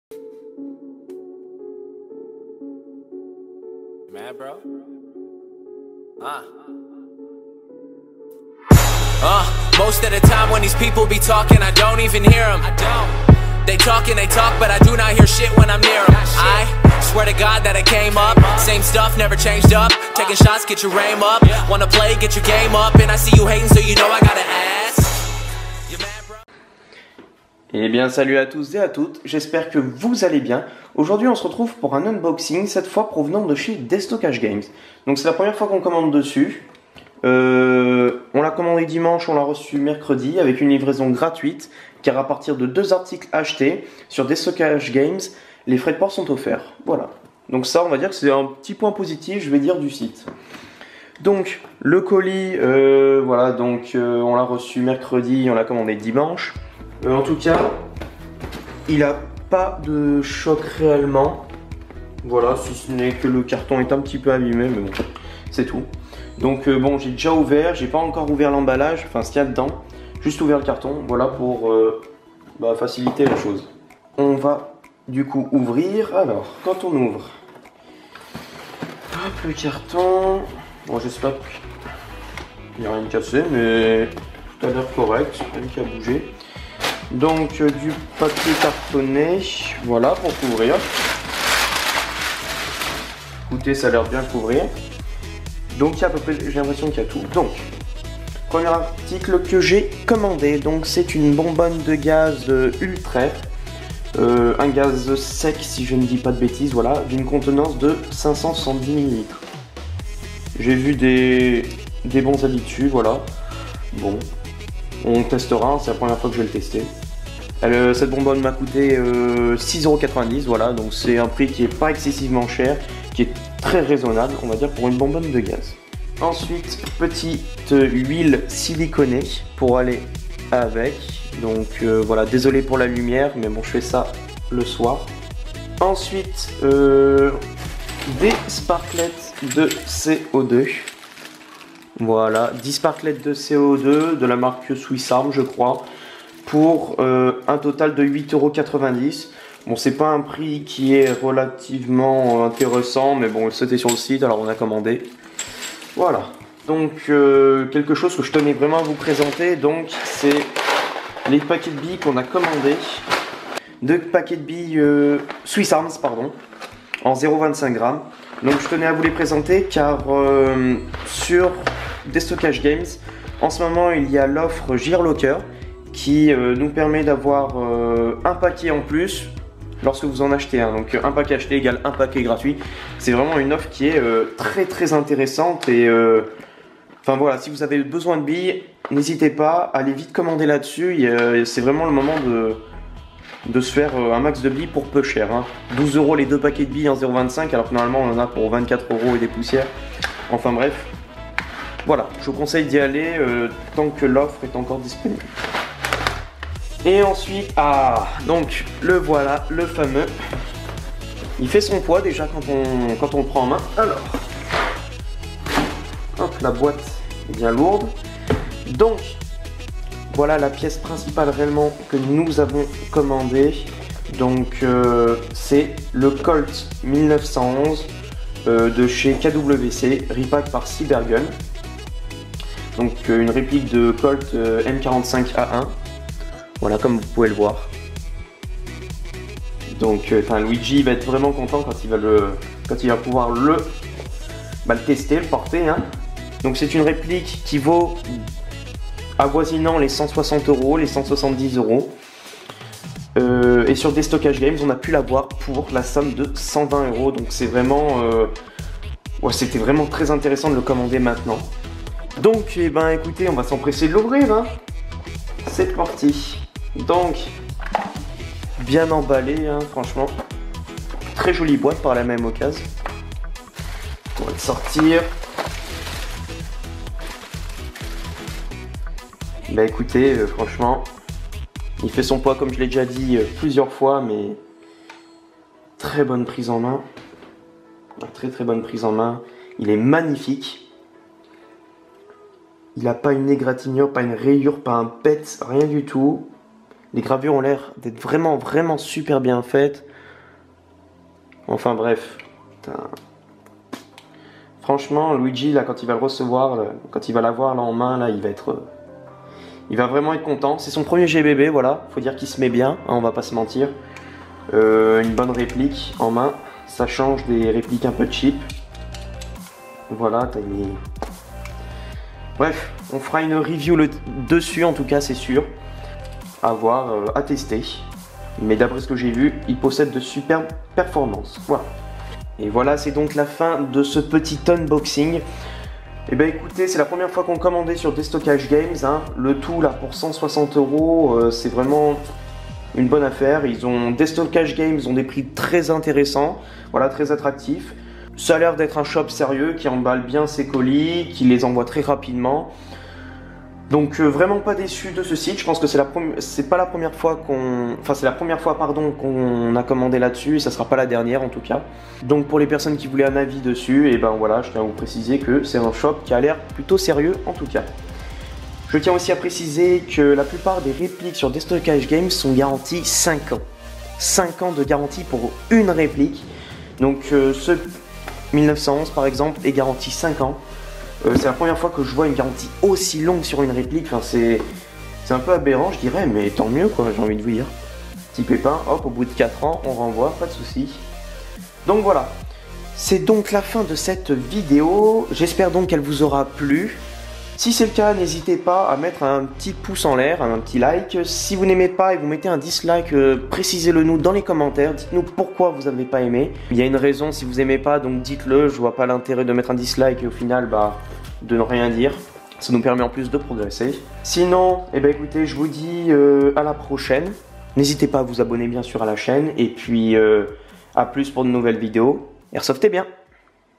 UNBOXING DESTOCKAGE GAMES / FIRST TIME
Salut les airsofteurs ou airsofteuses!! Petit unboxing airsoft cette fois-ci de Destockage games, nous espérons que cela vous plaira!;-)